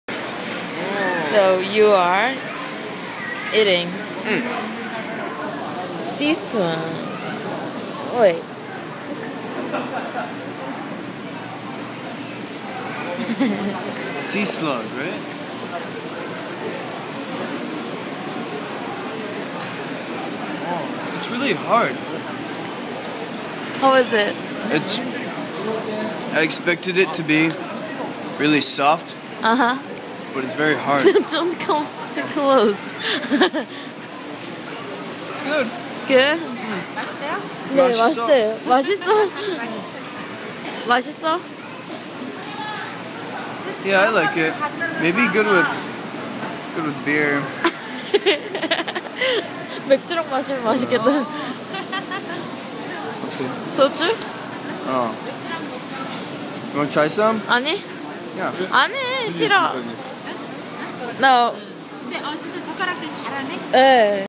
Eating sea slug in Korea
We went to noryangjin fish market is Seoul. I'd just eaten a whole love octopus and felt ready to take on a sea slug. It look squishy but was crunchy. Sea slug sashimi in hot sauce. A good drinking snack.